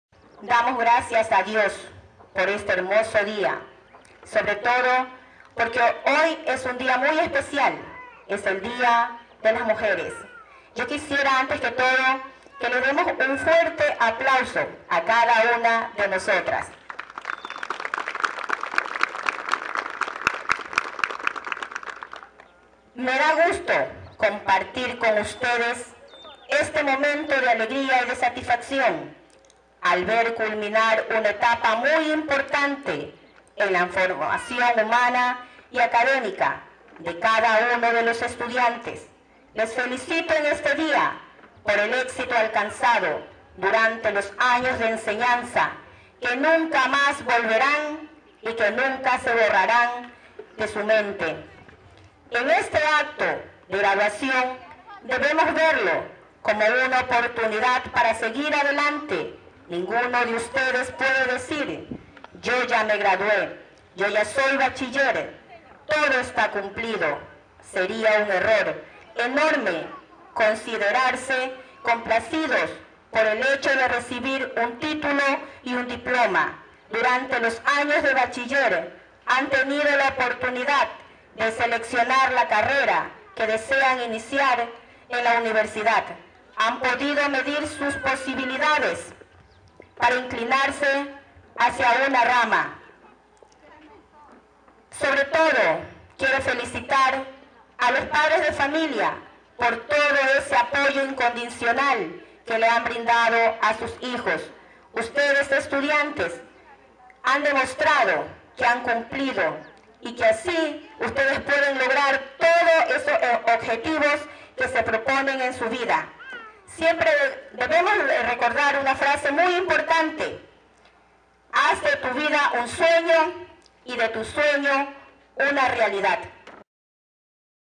LORENA GONZÁLEZ – DIRECTORA U.E CESAR QUIMIS
UNIDADES EDUCATIVAS FINALIZAN AÑO LECTIVO
CEREMONIA INCORPORACIÓN